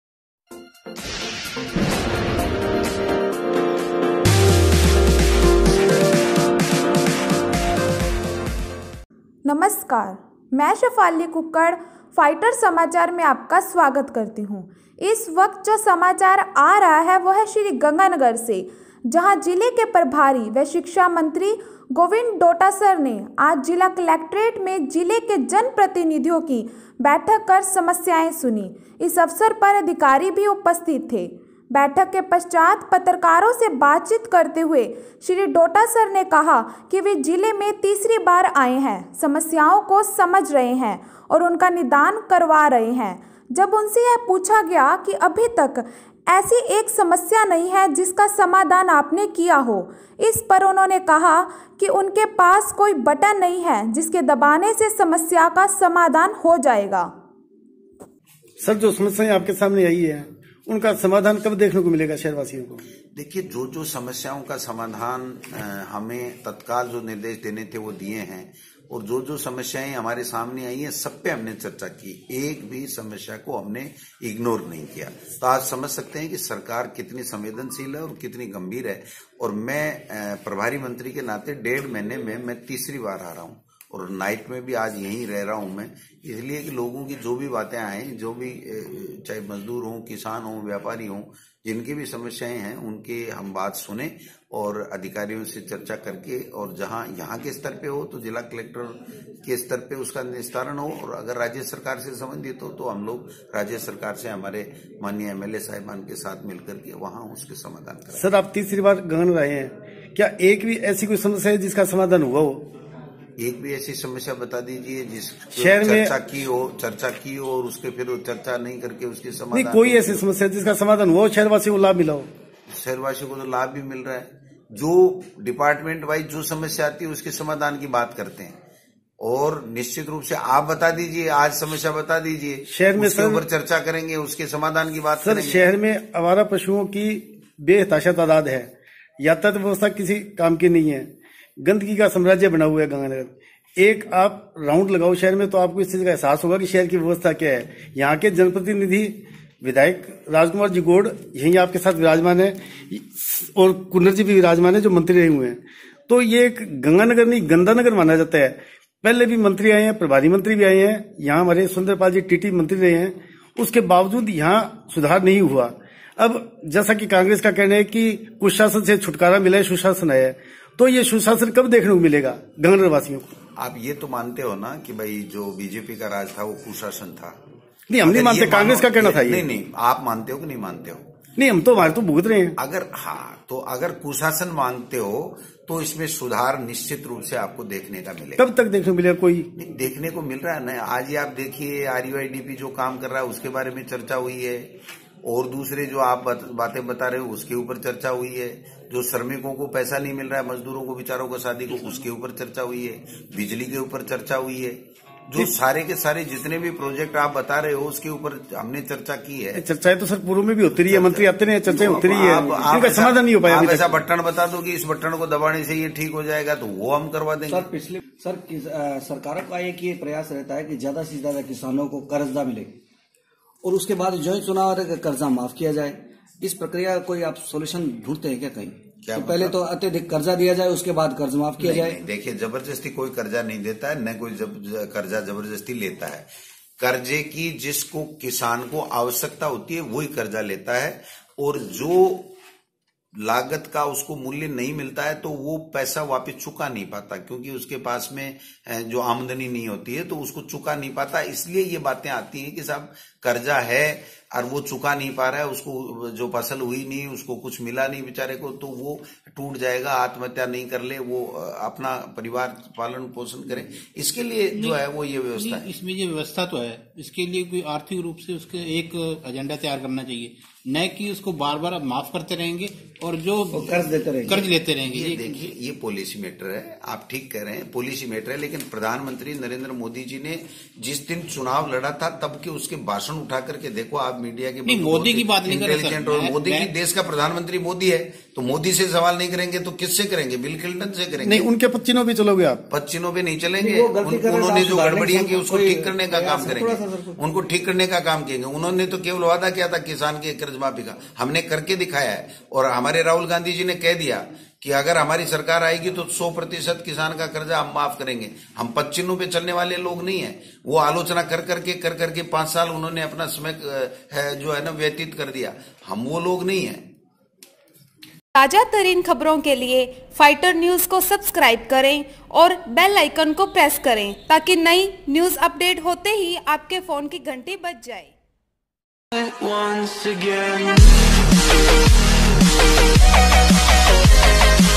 प्रभारी मंत्री का बयान , अगर समस्या सुलझाने का कोई बटन है तो वह बता दीजिये। Fighter News
Fighter News

About This Video :
इस वक्त जो समाचार आ रहा है वो है श्रीगंगानगर से जहां ज़िले के  प्रभारी व शिक्षा मंत्री गोविंद डोटासर ने आज ज़िला कॉलेक्टरटे में ज़िले के जनप्रतिनिधियों की बैठक कर समस्याएं सुनी ।
इस अवसर पर अधिकारी भी उपस्थित थे।
बैठक के पश्चात पत्रकारों से बातचीत करते हुए श्री डोटासर ने कहा कि वे ज़िले में तीसरी बार आये है, समस्याओं को समझ रहे है और उनका निदान करवा रहे है।
जब उनसे यह पूछा गया कि अभी तक ऐसी एक समस्या नही है जिसका समाधान आपने किया हो
 इस पर उन्होंने ने कहा कि उनके पास कोई बटन नही है जिसके दबाने से समस्या का समाधान हो जाएगा